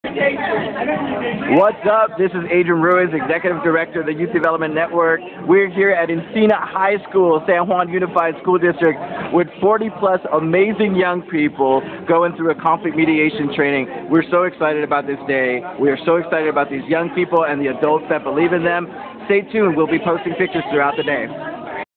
Encina High School Conflict Mediation Training
Youth Development Network facilitates the Encina High School Conflict Mediation Training 1/30/13!